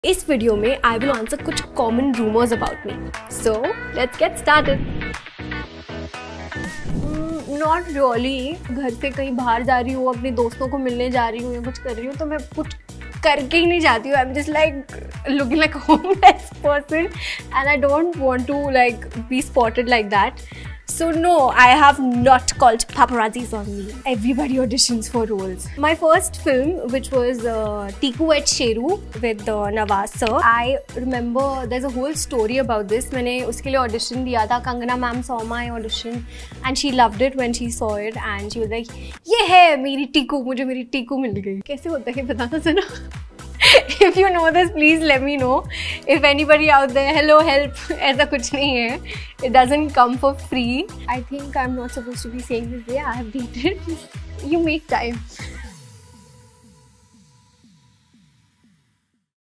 Busting Rumors With @AvneetKaurOfficials | Answering Assumptions | Get Ready with Nykaa
Join us as we ask Avneet Kaur to bust the rumors of celebrity life in this exciting teaser for GRWN! Watch to know the answers to the assumptions about Avneet Kaur while following her girl's night makeup routine in this candid and captivating . Get ready for an unfiltered and honest conversation with Avneet as she addresses the speculations and clears the air. Don't miss this exciting and engaging segment on Nykaa.